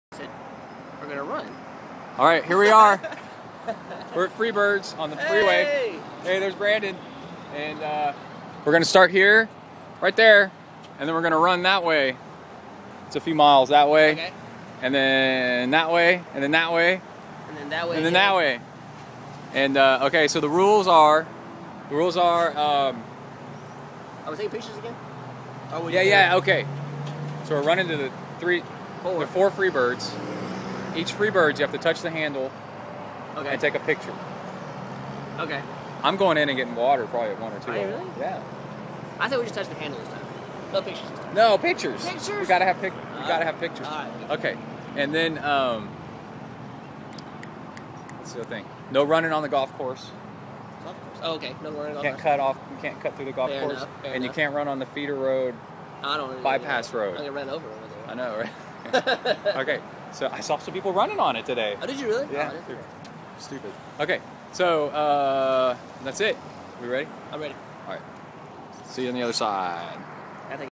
Start of Third Freebirds Burrito Run!
Brett and Brandon starting the 8.1 mile long run between all the Freebirds locations in College Station, TX.